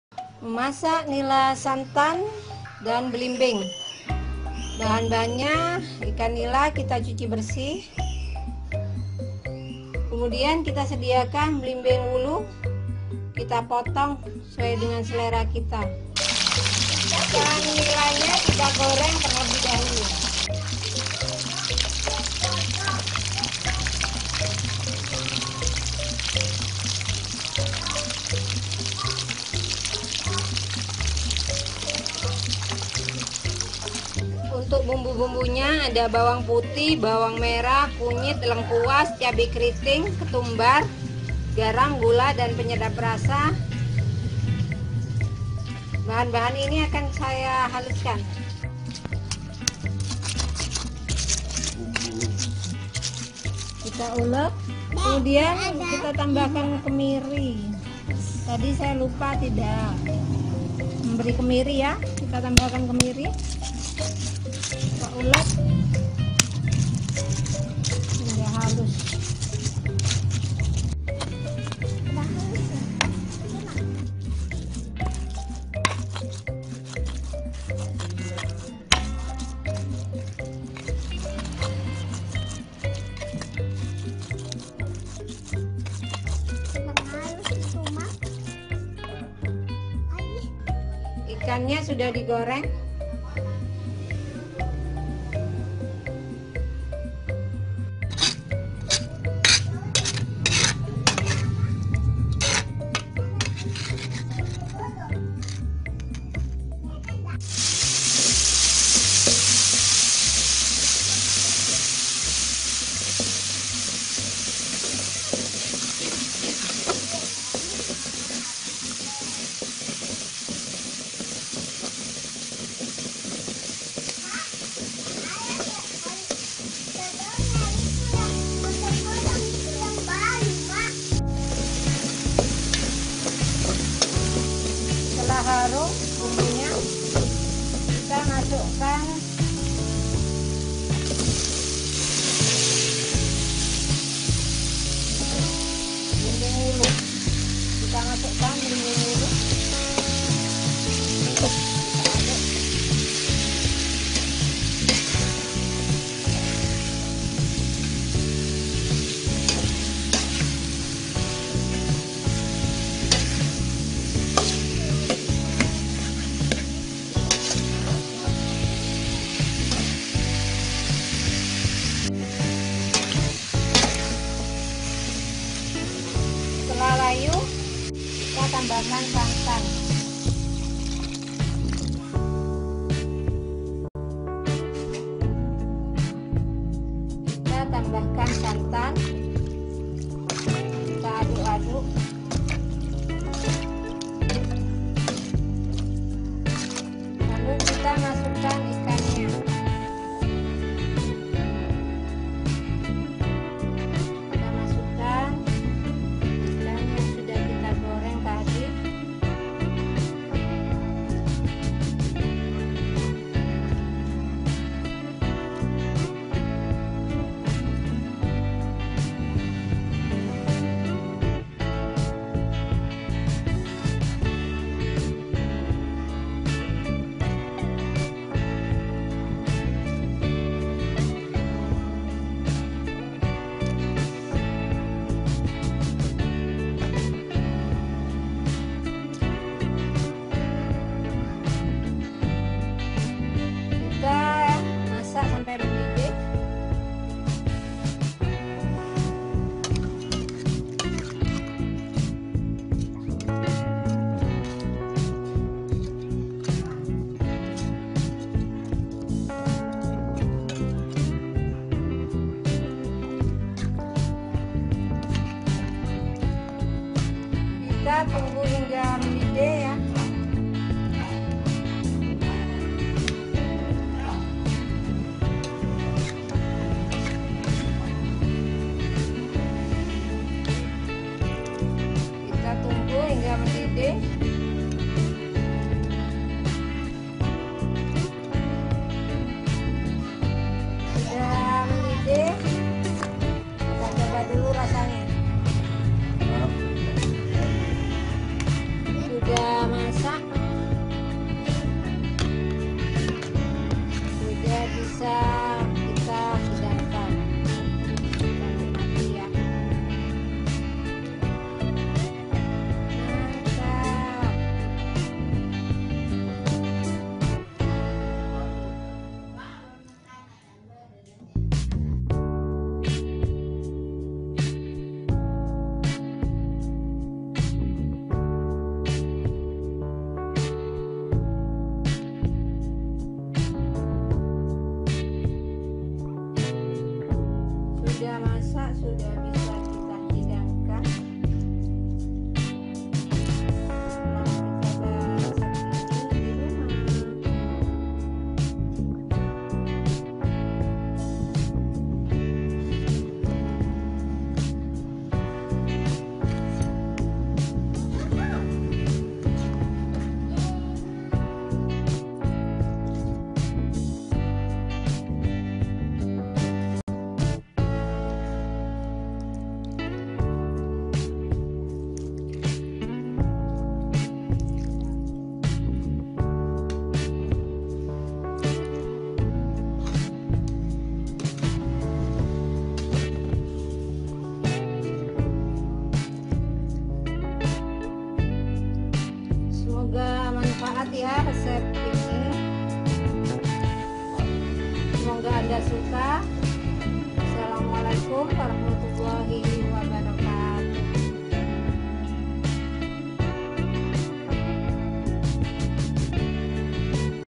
Resep Ikan Nila Blimbing Bumbu Kuning Maknyus Rasanya Nikmat Di Lidah!!
membuat olahan ikan bersama blimbing wulu dengan bumbu khas yang di ulek terasa menggoyang lidah aroma ikan goreng dan blimbing berpadu menjadi satu membuat masakan bumbu kuning yang lumer dan nikmat